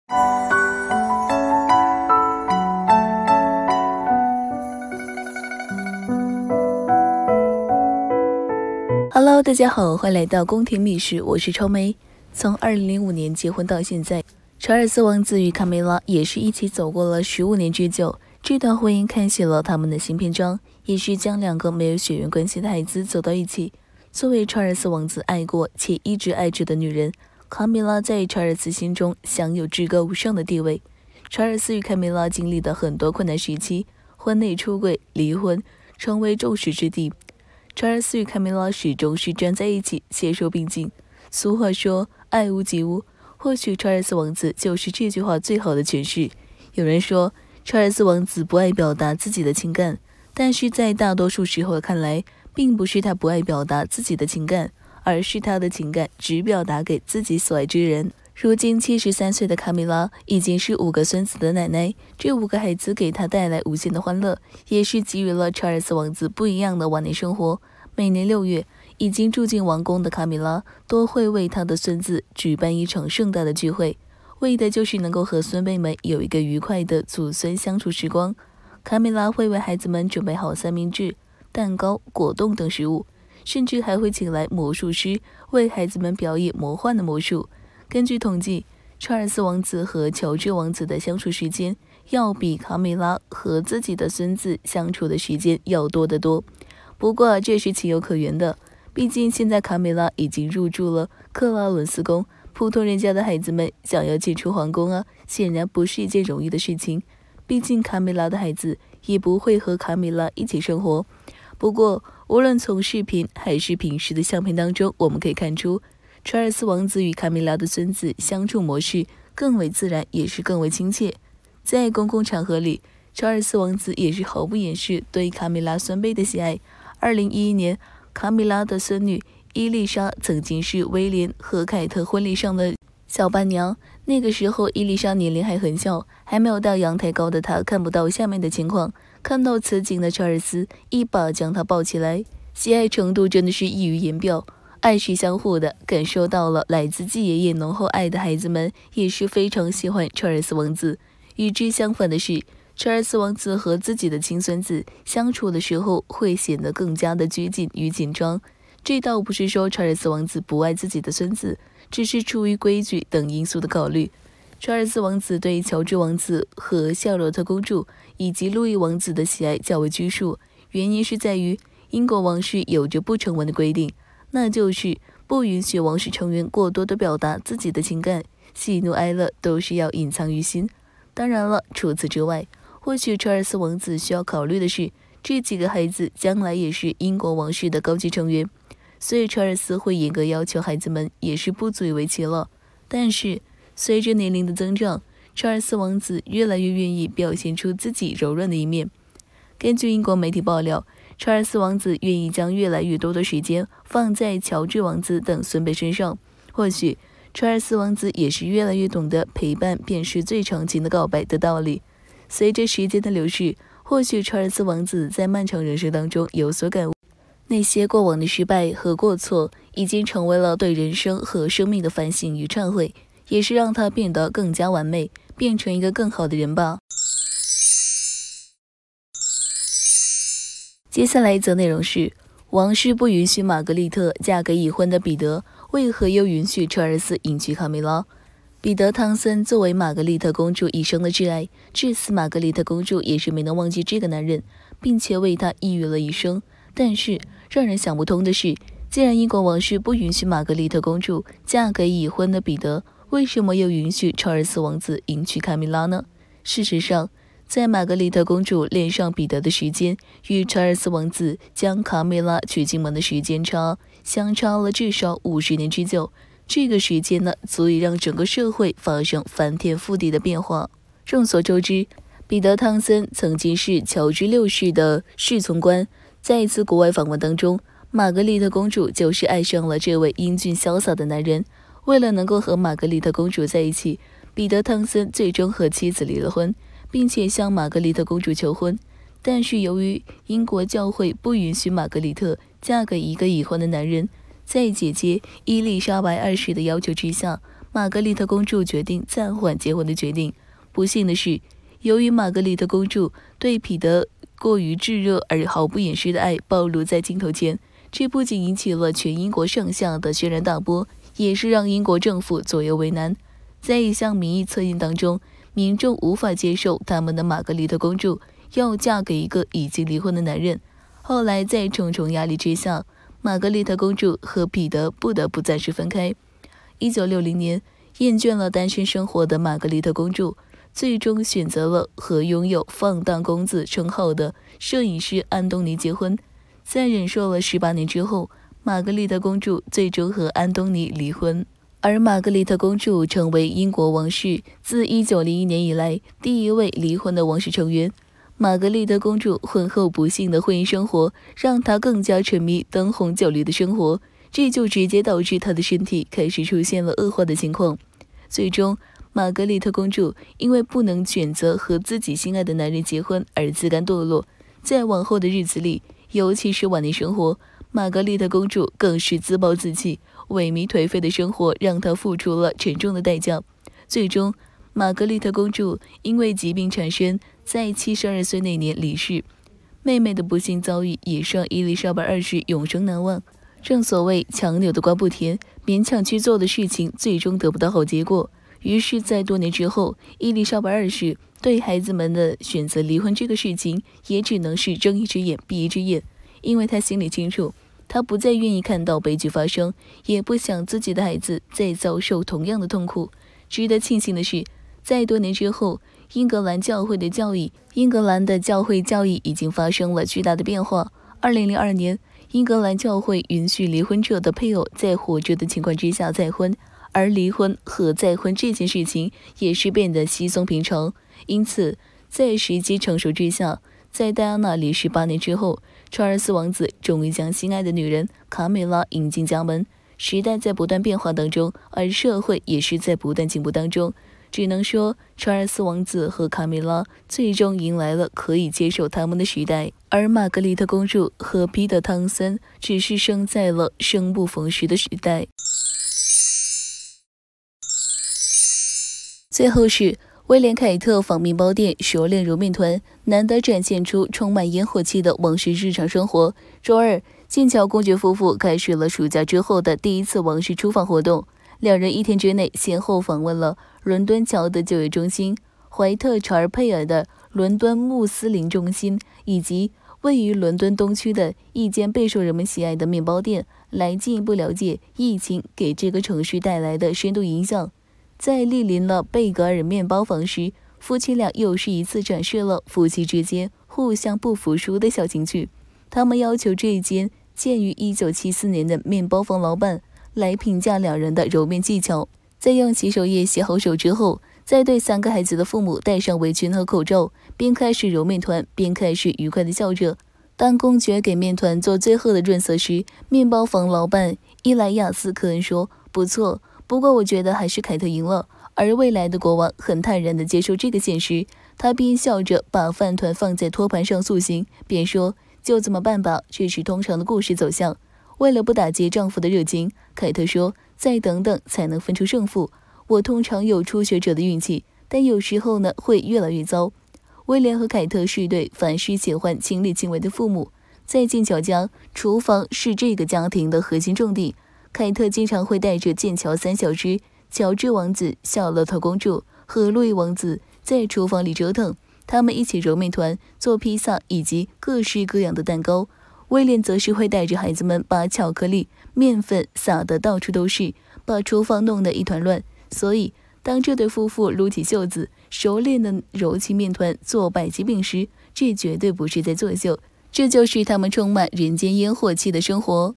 比起喬治王子，查爾斯王子更喜歡卡米拉的孫子，有這麼明顯嗎？｜宮廷秘史
比起喬治王子，查爾斯王子更喜歡卡米拉的孫子，有這麼明顯嗎？